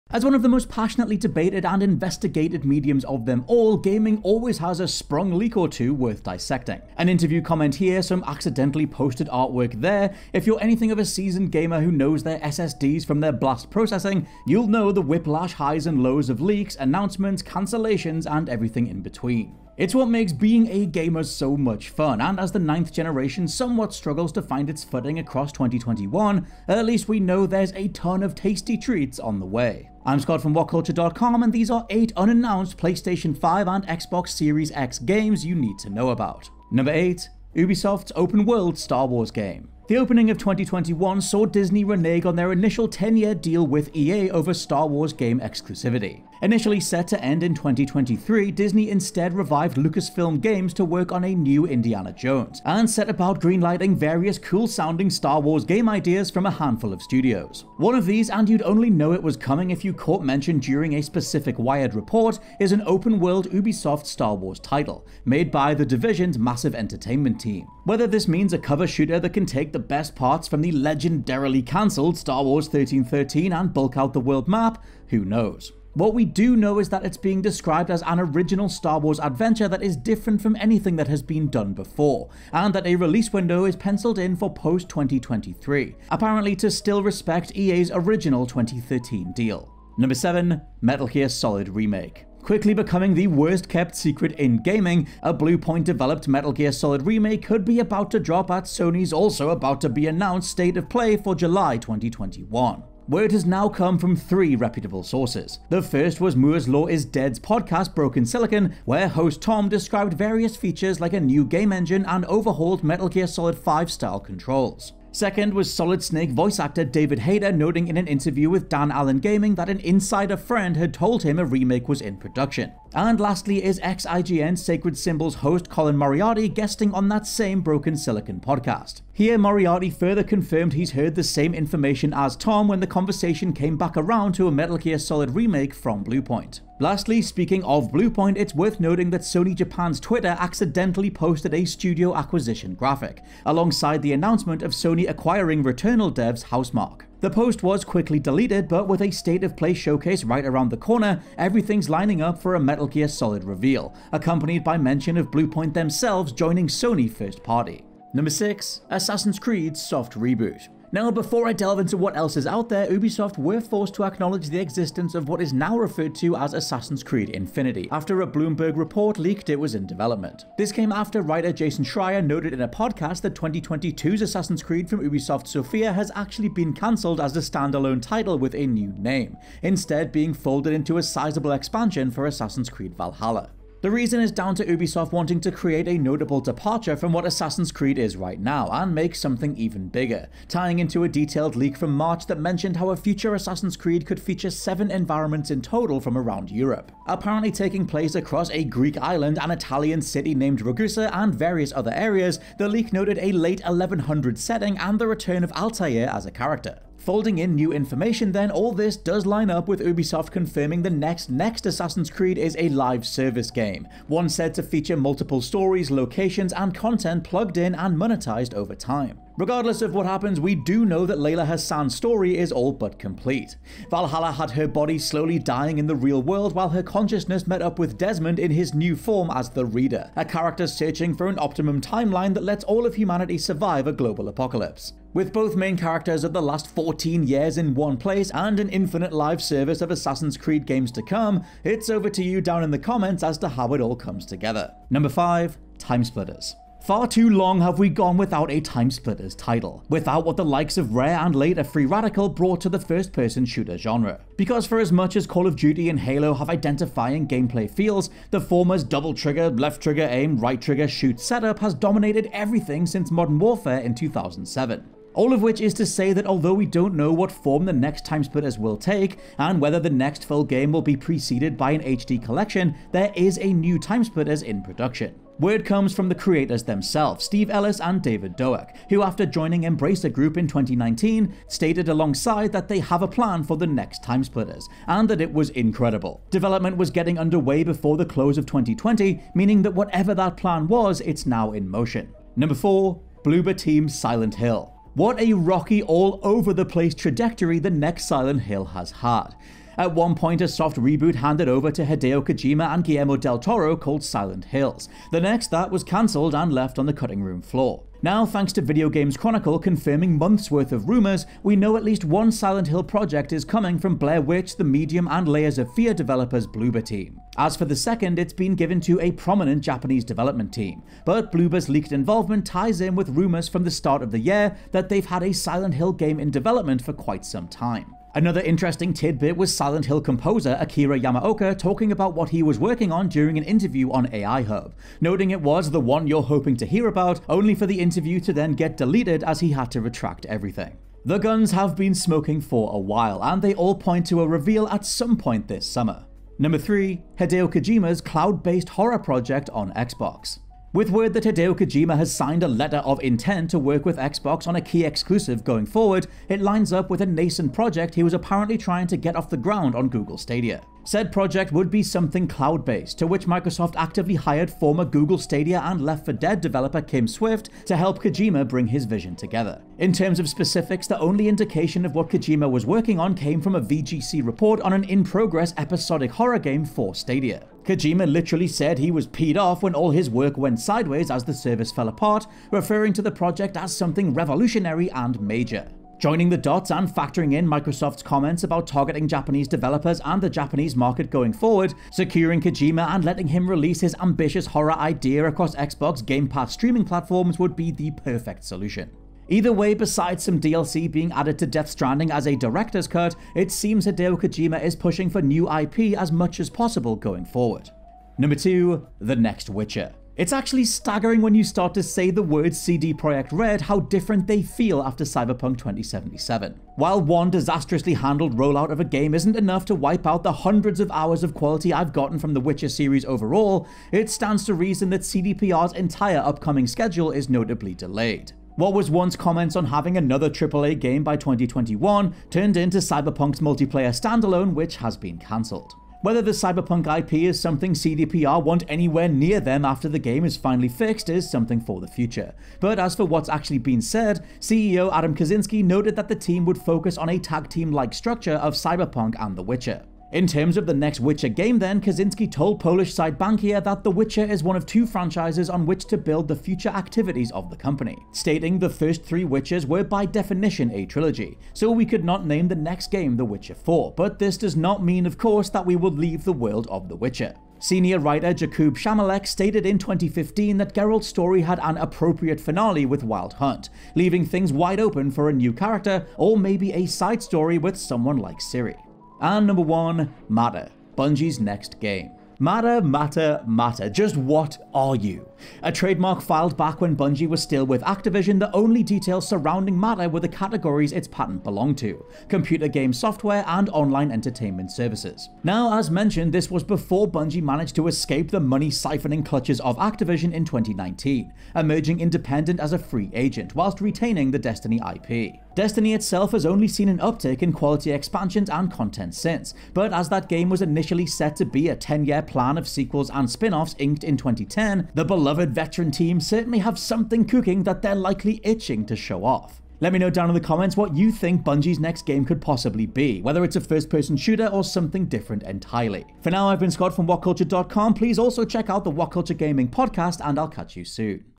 8 Unannounced Video Games You Need To Know About
Not one, but TWO Silent Hill games are in development.